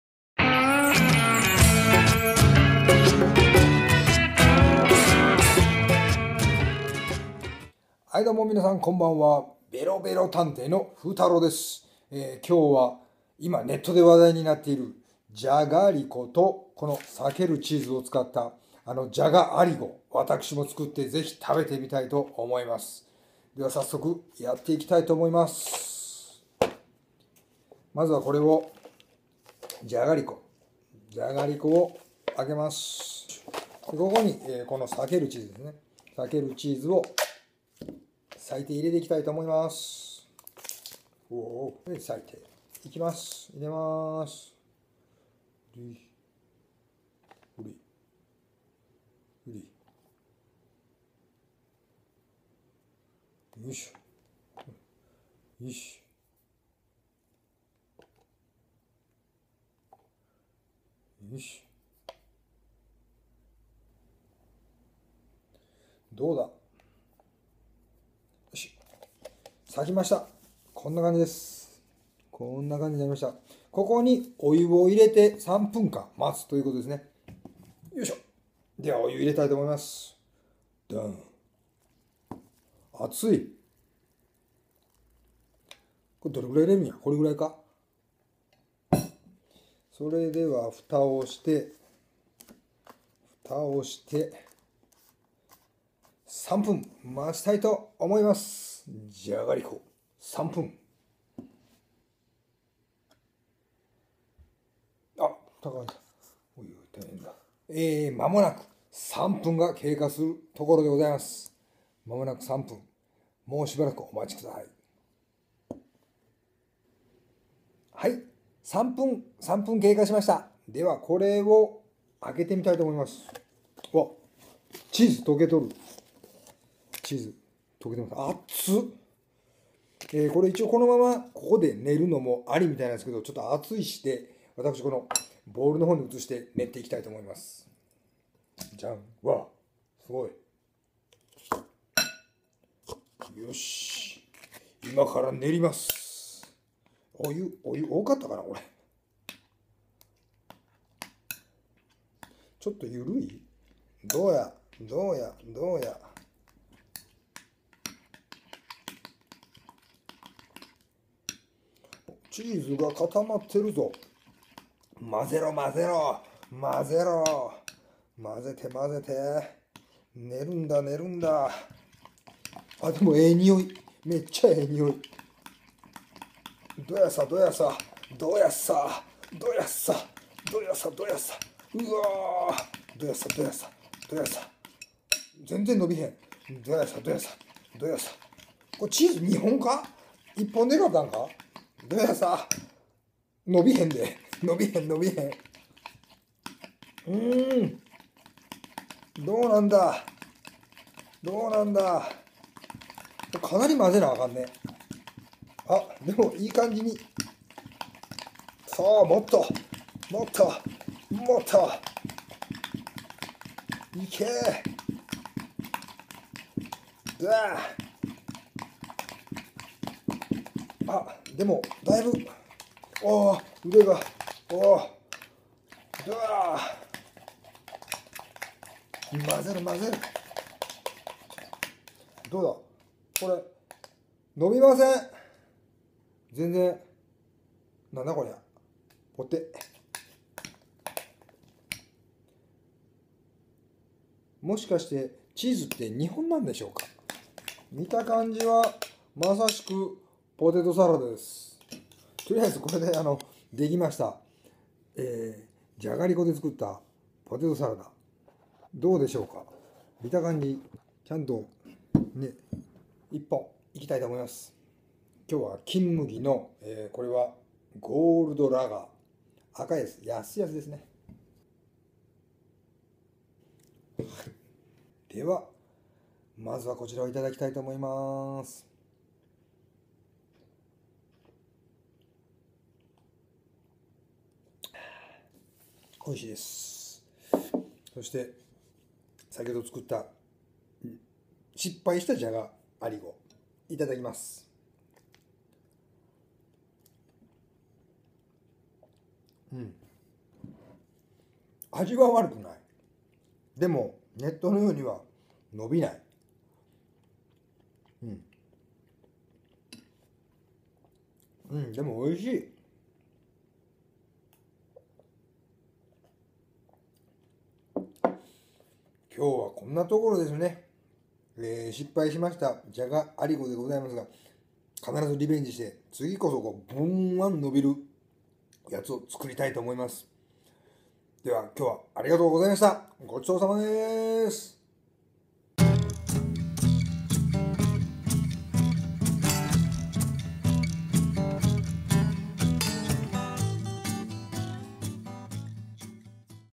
【家呑みべろべろ】じゃがアリゴを作ってみた！！
話題のじゃがアリゴ作りに挑戦してみましたが・・・。